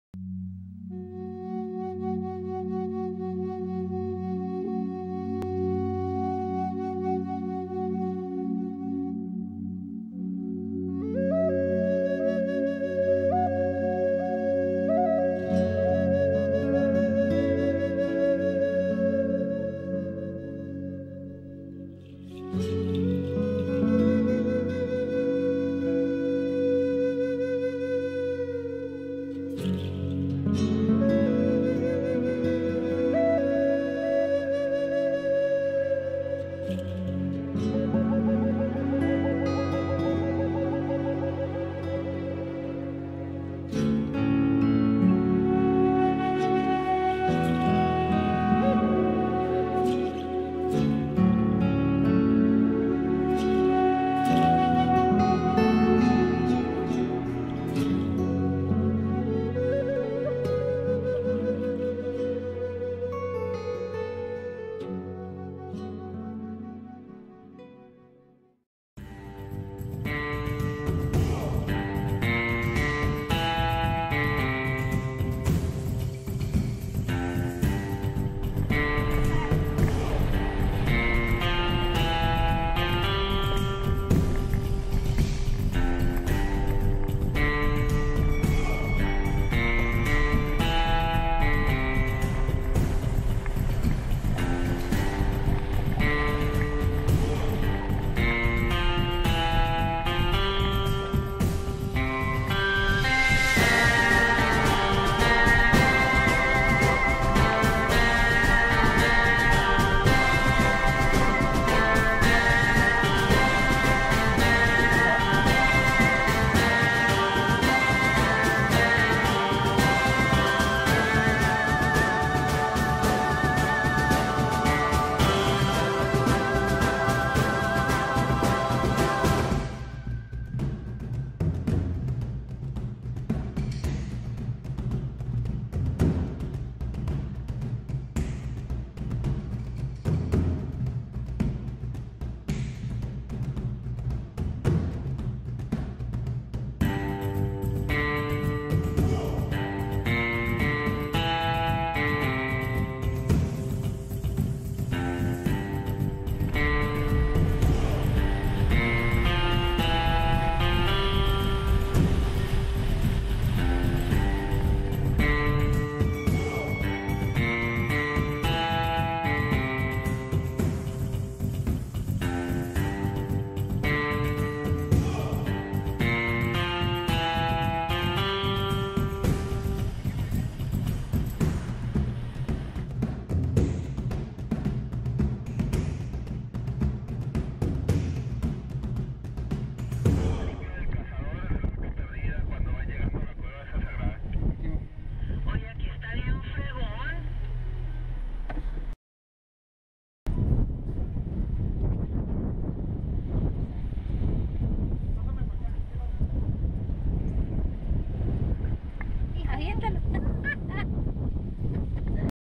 Ojo Caliente / Cañada Alamosa
Jeep trip to Ojo Caliente (Warm Spring) / Cañada Alamosa New Mexico.